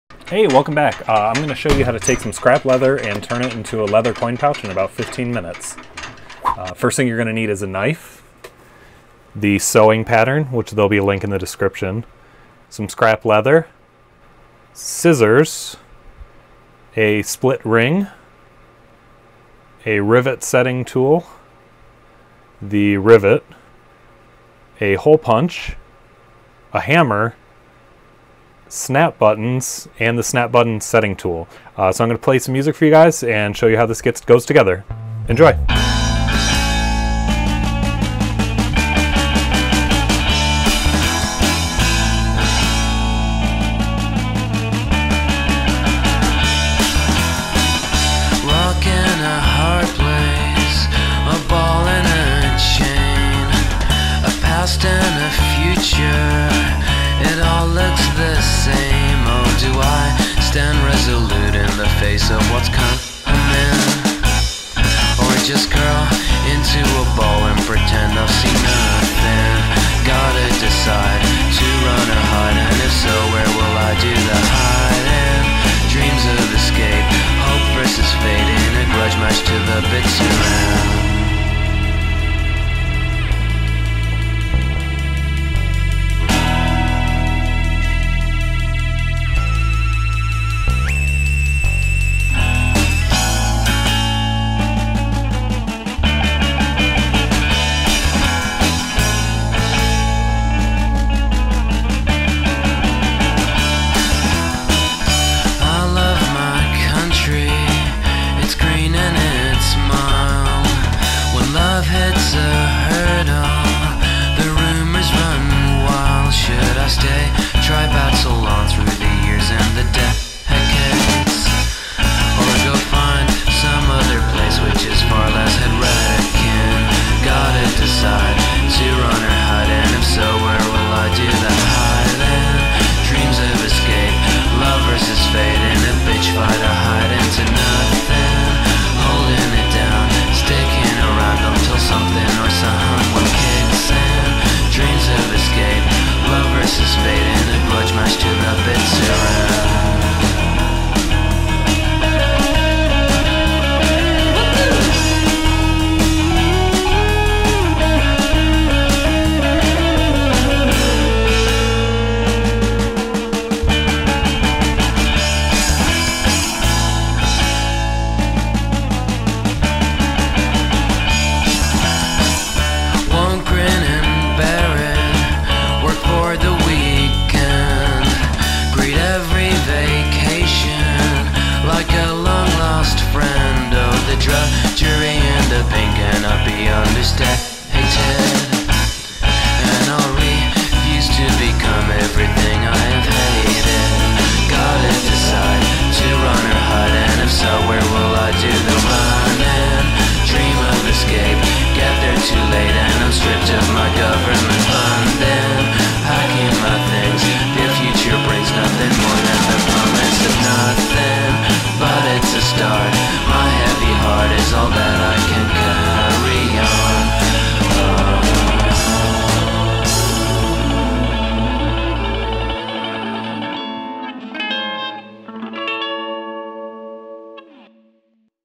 Scrap leather coin pouch - LMSAT Quick Project
I've got a bunch of leftover leather that isn't good for much, if you've got the same problem, follow this video to see how you can use it up! This pattern will work with just about any thick fabric.

This is one of a few products I'll be selling at the PFC Night Market on Thursday, Sept 15 in Kalamazoo, MI.

THANKS FOR TAKING THE TIME TO WATCH!